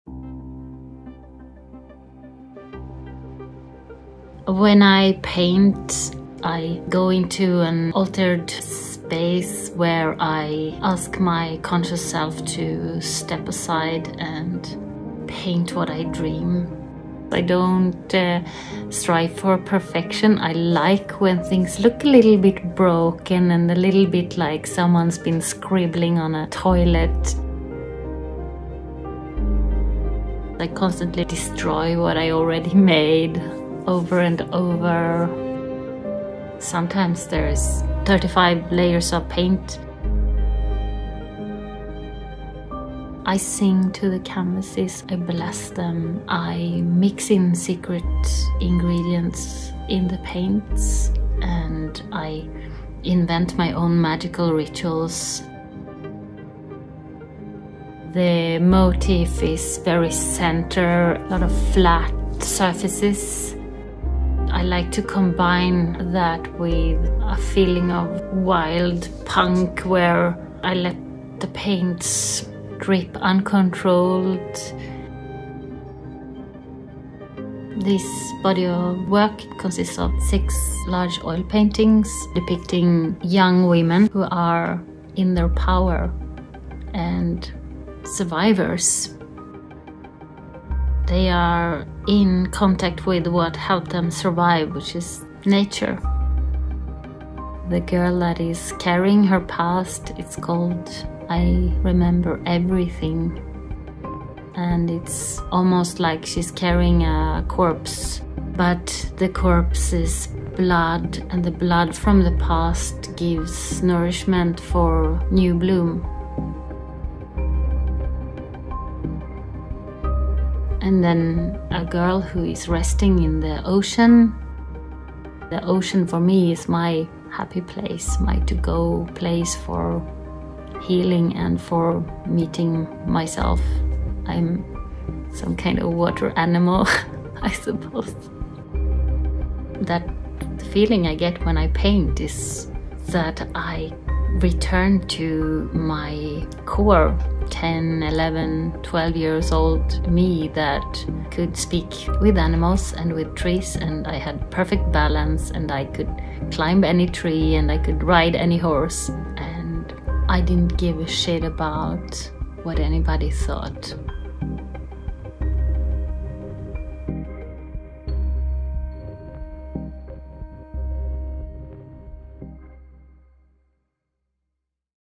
AK von Malmborg on painting Survivors
AK von Malmborg describing the creative process painting the series of survivors titled The Beauty and The Terror
The title of this new body of work comes from Rilke's poem, Go to The Limits of Your Longing, where despite the beauty and the terror, he says "Just keep going. No feeling is final."
Knowing my own traumatic experiences, and the similar experiences of women; these paintings serve as a visual feminist manifesto for the survivor. Addressing both the terror of trauma, and the beauty of what helps you survive.
Blood from the past gives life to new bloom and the pre-pubescent girls are healed by their spirit-connection with nature. Their nudity is not sensationalised, nor does it invite or permit a gaze of concern, pity or objectification - they stand proud in their autonomy, devoid of the need of adult interference. They are, in their nakedness, in a place of total authenticity. Comforted, protected and given strength by the elements of nature that join her, the tale of survivorship is not a path walked alone.
My painting process employs performative practices through making magic rituals, I have sung to the canvases, mixed sacred ingredients in the paint and cried many tears whilst painting them. They are full of secret blessings and hidden messages in layers of oil paint.
Go to the Limits of Your Longing by Rainer Maria Rilke, translated by Joanna Macy

Listen

God speaks to each of us as he makes us,
then walks with us silently out of the night.

These are the words we dimly hear:

You, sent out beyond your recall,
go to the limits of your longing.
Embody me.

Flare up like a flame
and make big shadows I can move in.

Let everything happen to you: beauty and terror.
Just keep going. No feeling is final.
Don’t let yourself lose me.

Nearby is the country they call life.
You will know it by its seriousness.

Special thanks to Robin Siwe editor, Anna Zackrisson interviewer, Mats Skagerström Ågren camera, Rikard Borggård music and Ida Cassar for invaluable help with words and thoughts. I love you.